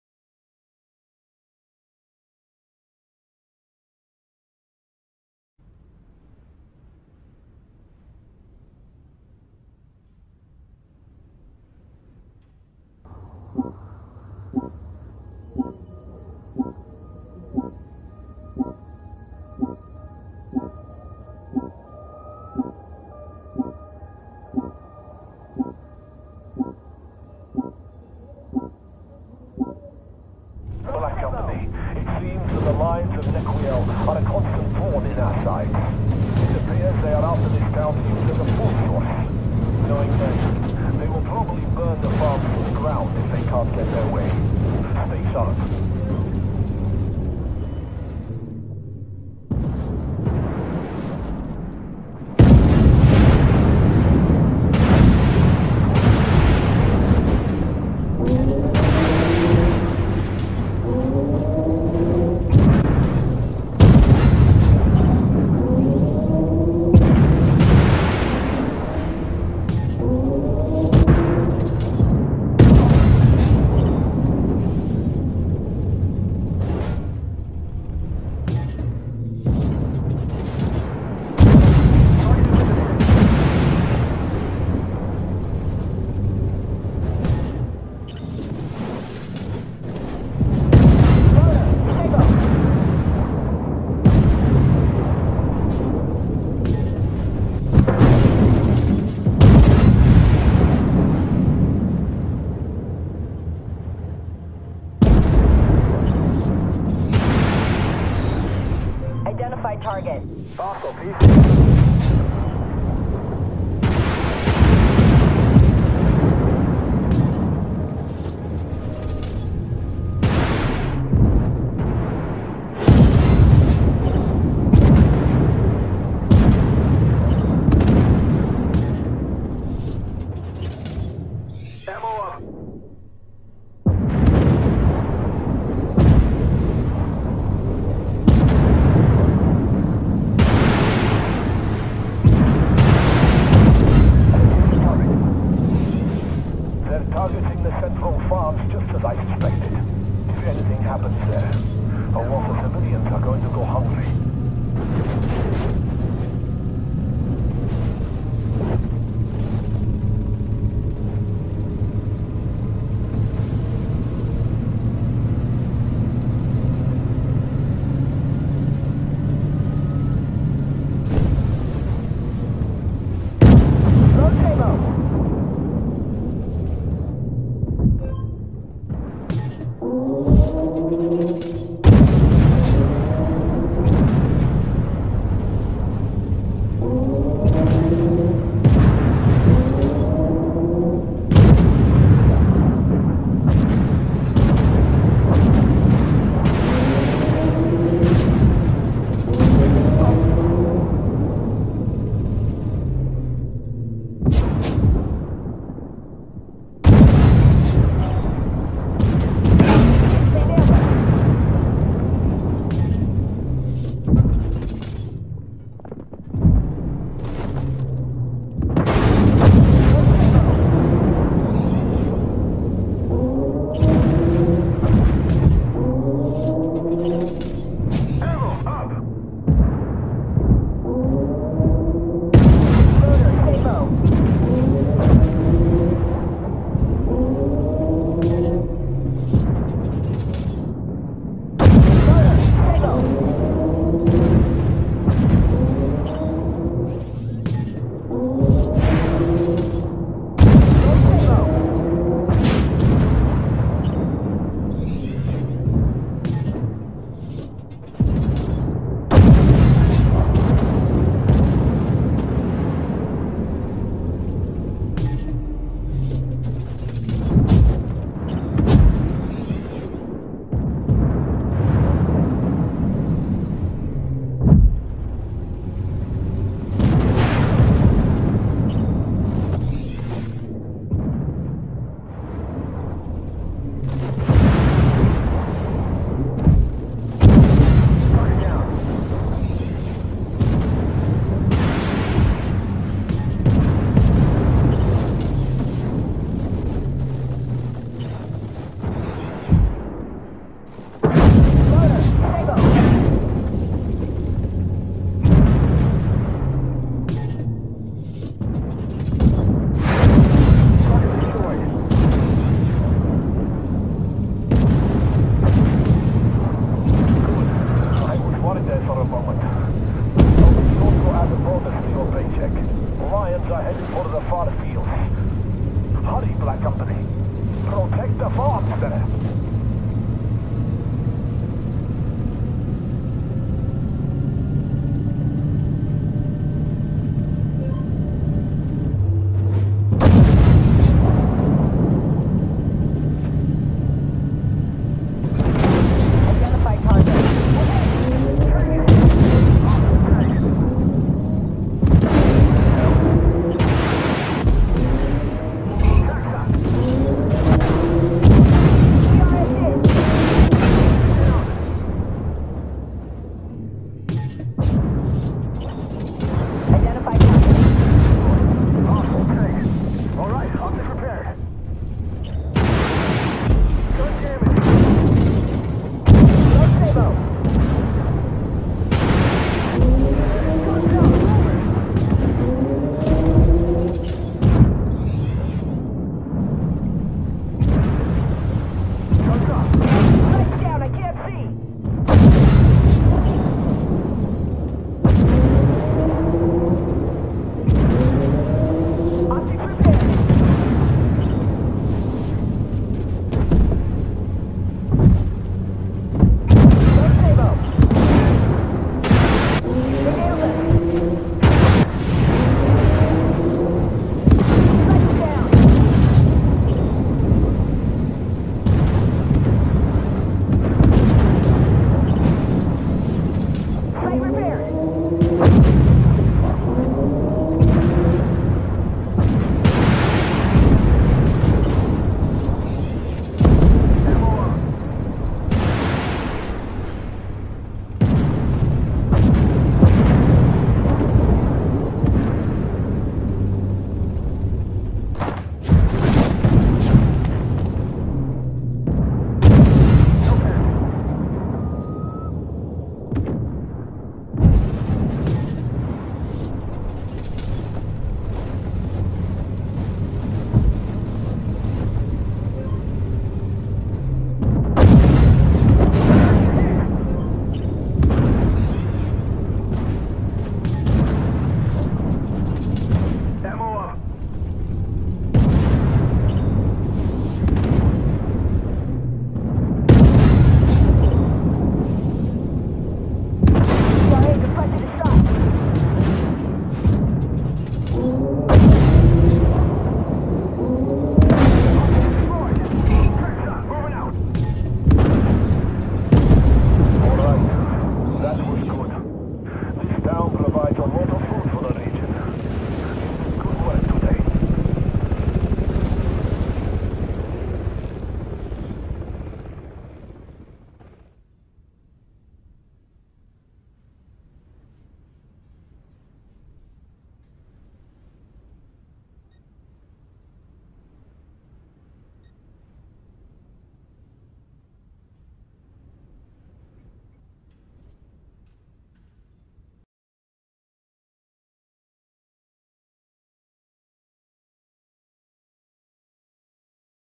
Update 0.20 PVE Game Play - M1A2 Abrams MBT on Starry Night
Most games are in PVE mode unless other wise specified. Today's game is with the Tier 8 M1A2 Abrams MBT on Starry Night.